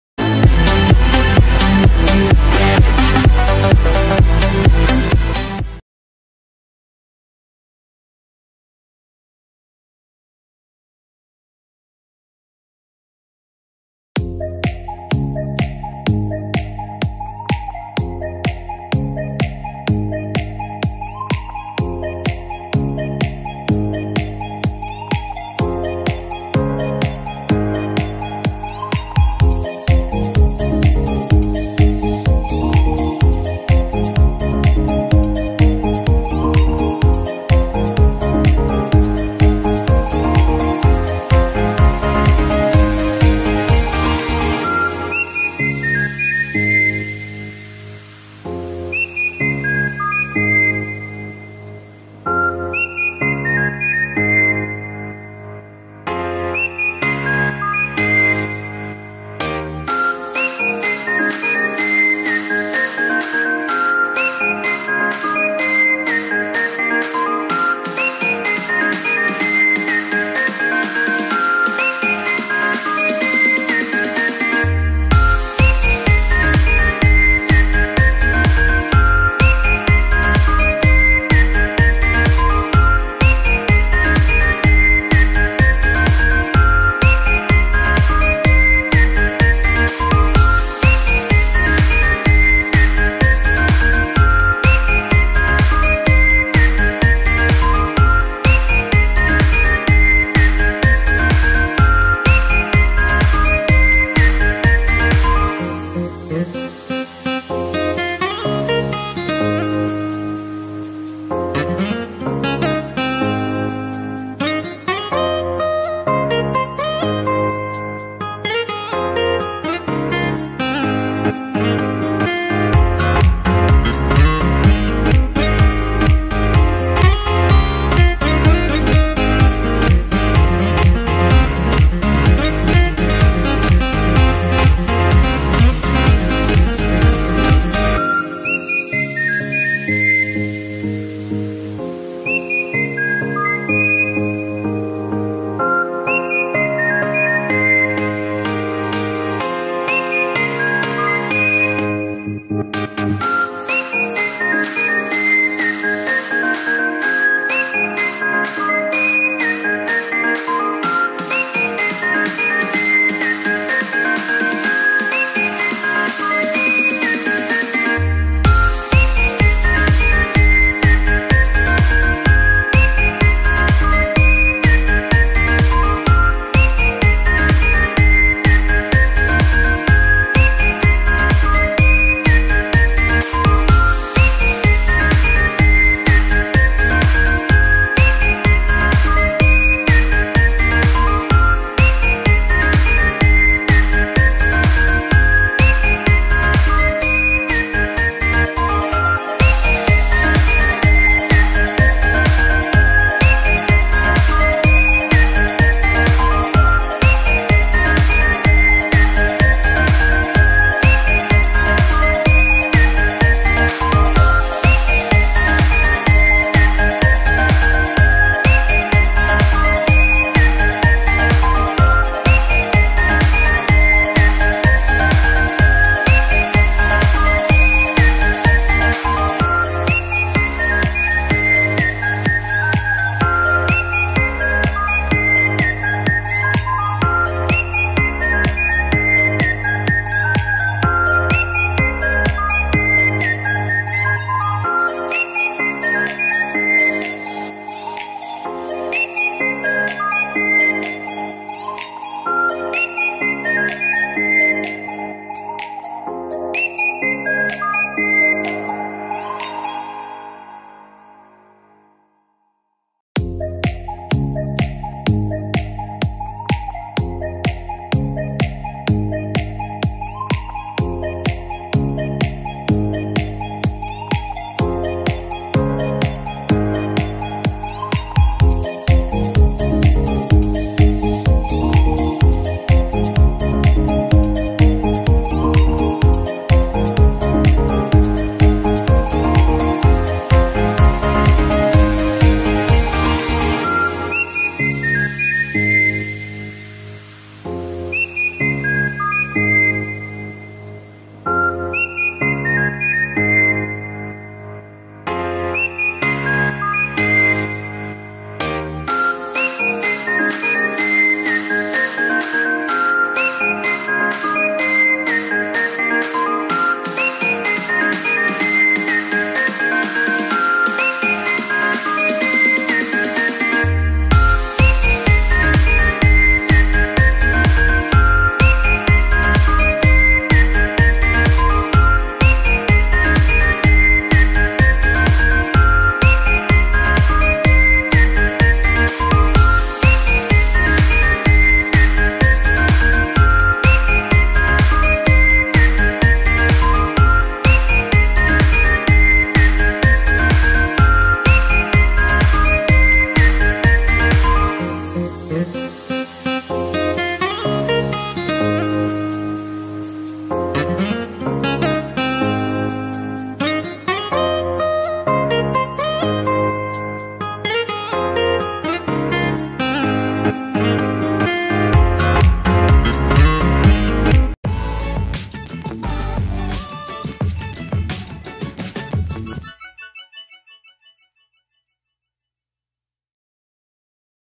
Amazing peacock dance | Peacock dance compilation | Animal Zootopia
The Indian peafowl, also known as the common peafowl, and blue peafowl, is a peafowl species native to the Indian subcontinent. It has been introduced to many other countries. No matter the species of peacock, these colorful creatures boast impressively sized and patterned plumage that they fan out for display purposes. It isn't an act of vanity, though -- peacocks fan out their feathers as part of a courtship ritual to attract a mate.